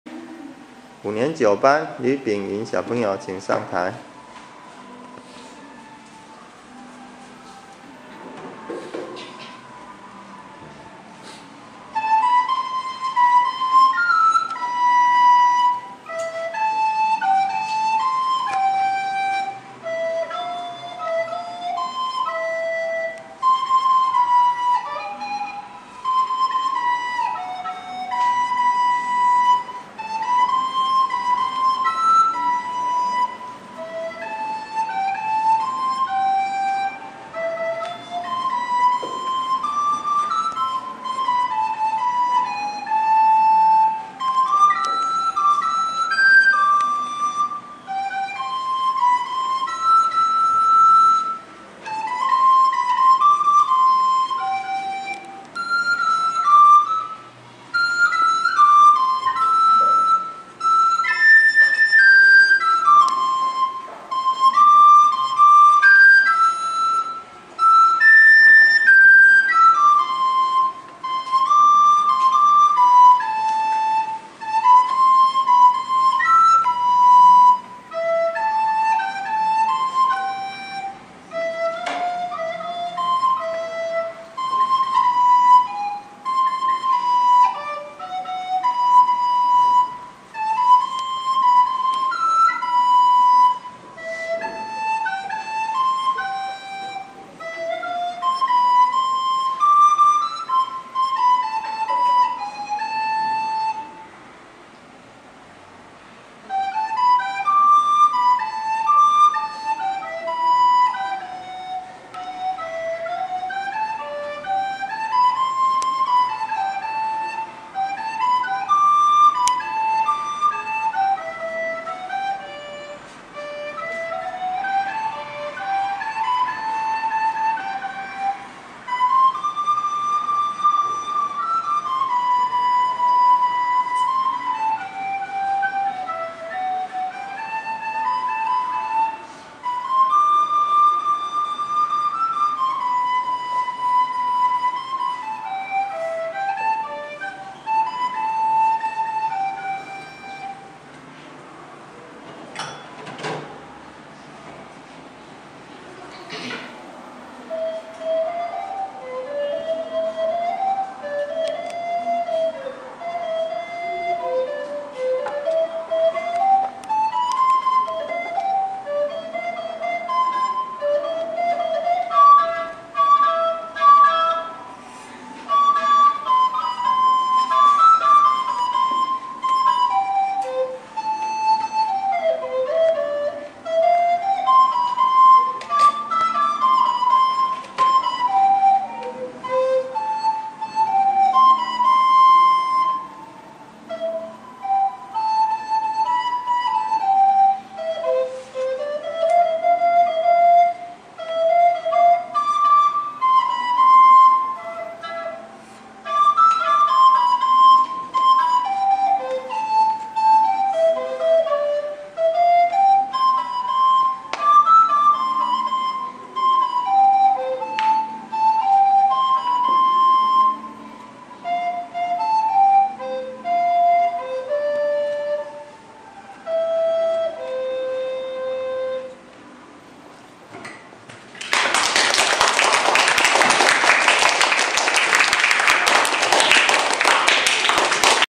台南市東區復興國小509呂秉耘 直笛2011 12
2011台南市東區復興國小五年級個人音樂表演MV

2011台南市東區復興國小弦樂團音樂活動&個別專輯MV